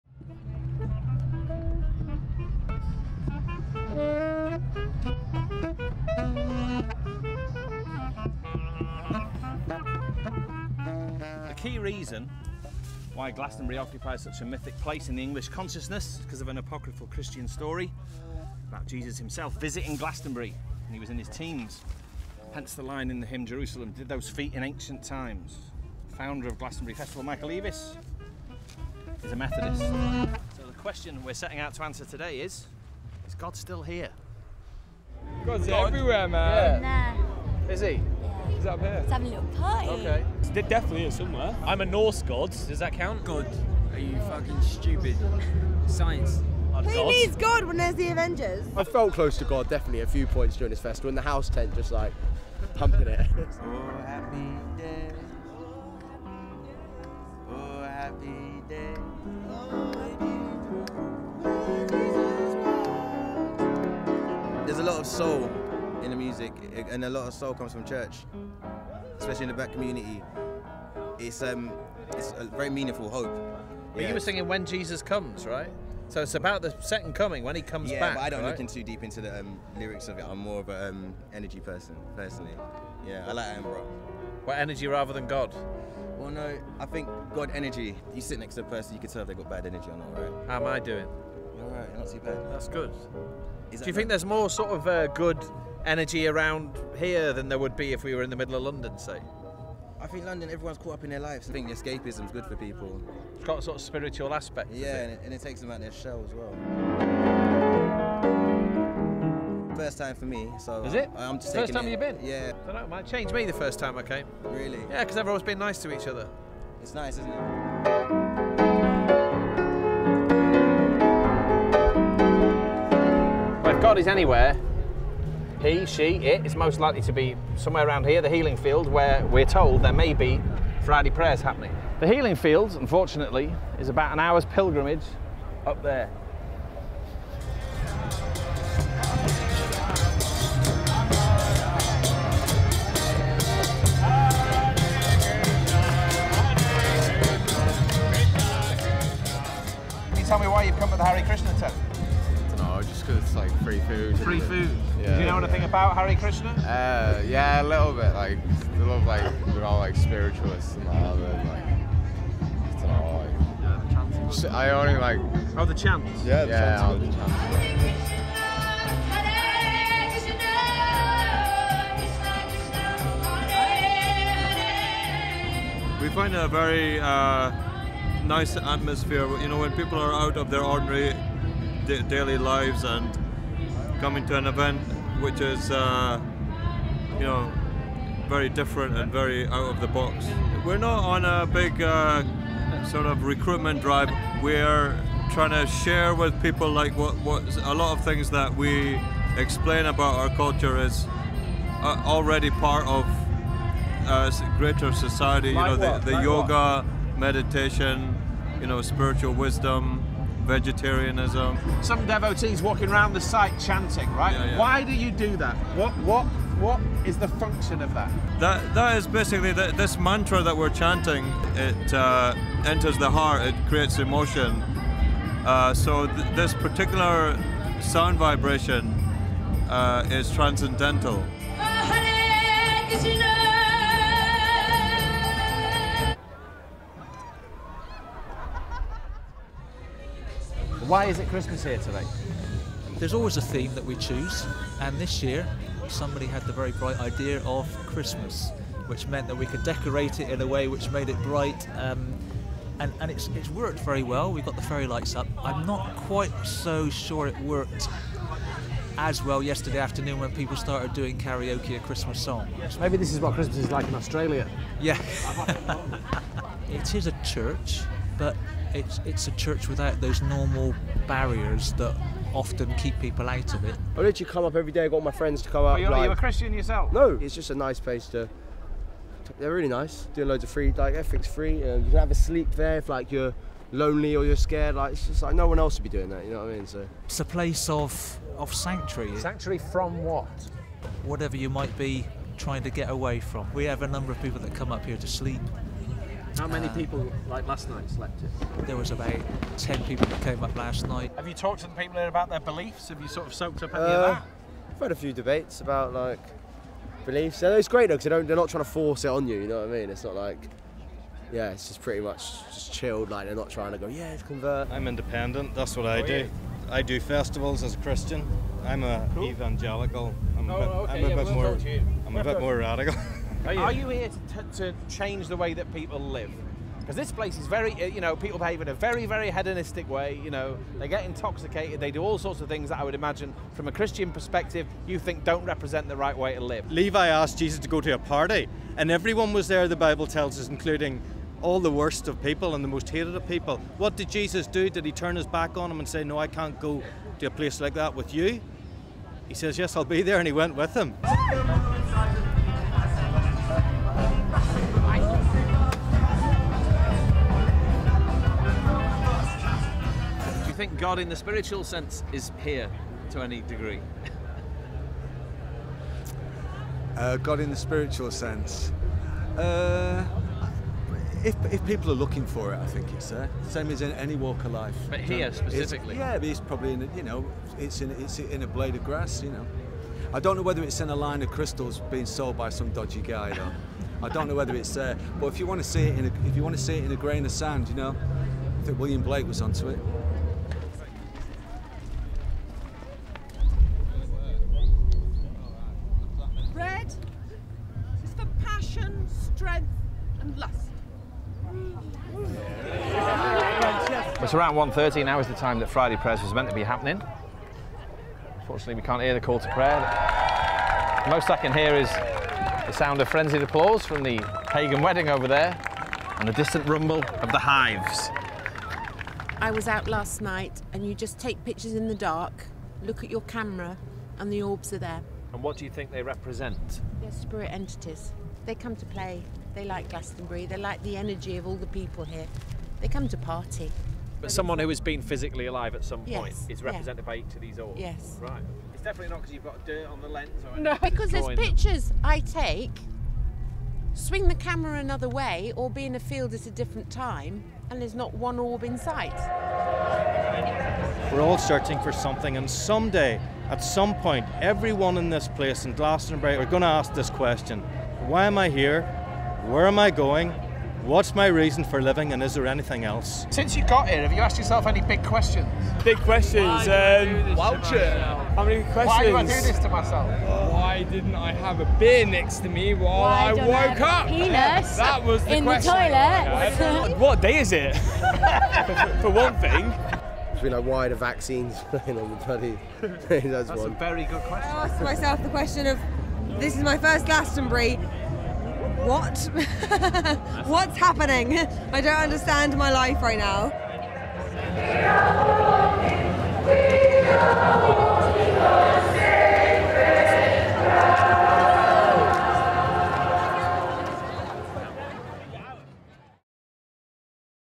Glastonbury 2013: is God at Glastonbury?
In among the hedonism at Glastonbury 2013, John Harris and John Domokos go rummaging through sacred Somerset's religious and spiritual undergrowth. What's here? Christianity, Hare Krishna, pagan weddings, and people who think Glasto is haunted by "orbs". They also ask the weekend's big questions: who are we? Why are we here? And why are the Vaccines on the Pyramid stage?